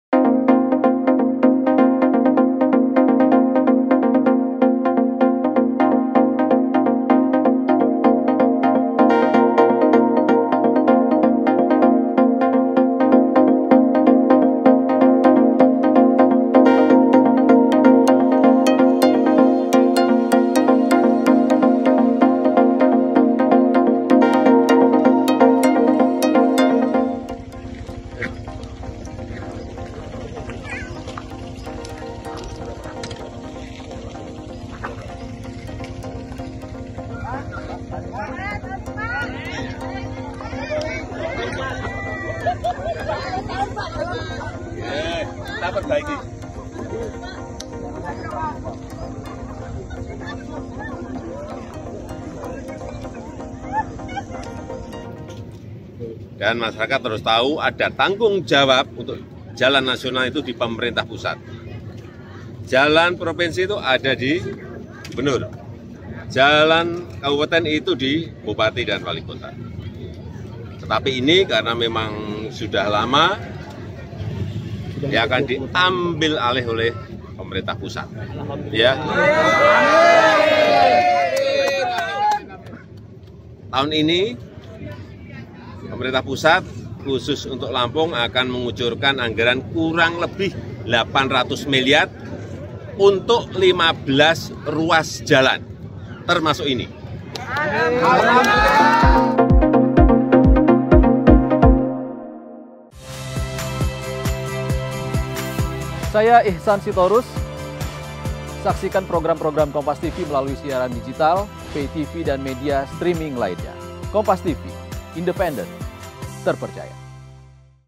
Gubernur Lampung Tepuk Tangan dan Ucap Syukur Saat Jokowi Ambil Alih Perbaikan Jalan Rusak
LAMPUNG, KOMPASTV - Gubenur Lampung Arinal Djunaidi tepuk tangan saat Jokowi mengatakan Pemerintah Pusat bakal ambil alih sebagian perbaikan jalan rusak di Lampung.

Ia juga mengucap syukur saat tahu pemerintah pusat akan menggelontorkan uang ratusan miliar untuk pekerjaan tersebut.

Presiden Joko Widodo atau Jokowi menyebut Pemerintah Pusat akan mengambil alih perbaikan sejumlah jalan rusak di Lampung.

Hal tersebut Jokowi sampaikan di hadapan Gubernur Lampung, usai bersama jajaran meninjau kondisi jalan rusak di Lampung.

"Ini (jalan rusak) kaena memang sudah lama, akan diabil alih oleh pemerintah pusat," ujar Jokowi di Lampung, Jumat (5/5).

"Tapi juga ada beberapa ruas yang jadi tanggung jawab pak Gubernur, jadi tanggung jawab Bupati. Jangan semuanya pemerintah pusat," lanjutnya.

Hal tersebut langsung disambut tepuk tangan bahagia Gubernur Lampung Arinal Djunaidi.

Baca Juga [FULL] Tanggapan Presiden Jokowi Setelah Merasakan dan Meninjau Langsung Kondisi Jalan Lampung di